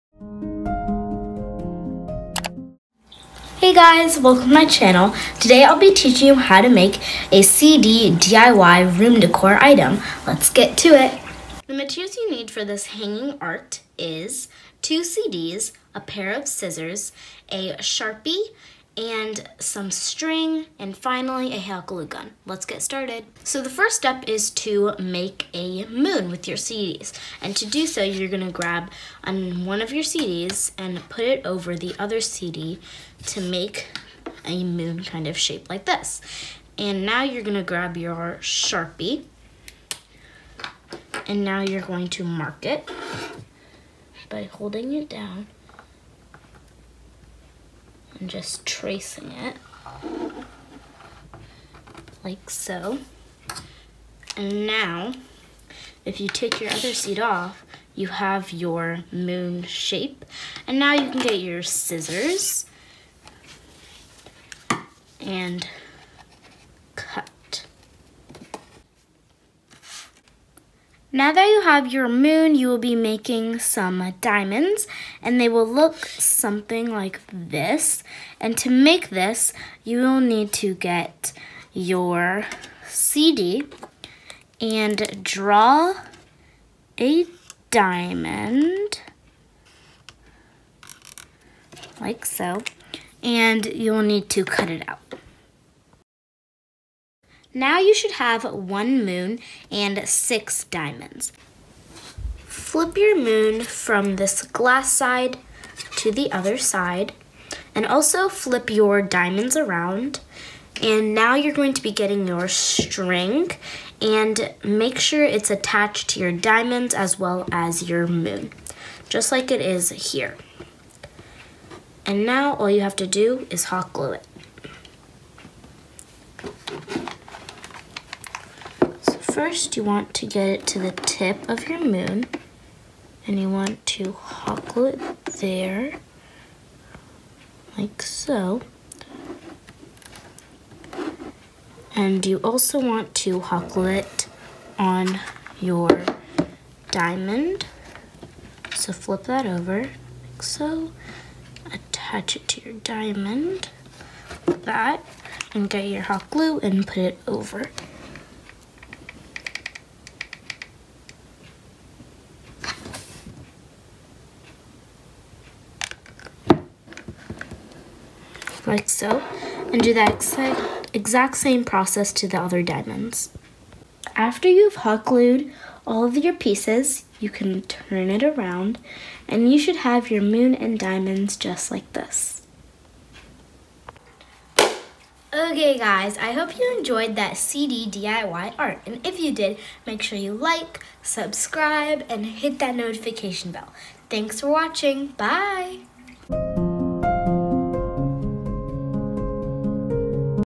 DIY MOON Wall Decor Out of CDs!
Hey guys! In this video, I will be teaching you how to make this cute wall decor only out of CDs!

✰ editor/videographer ✰
Joshika Sathyamathan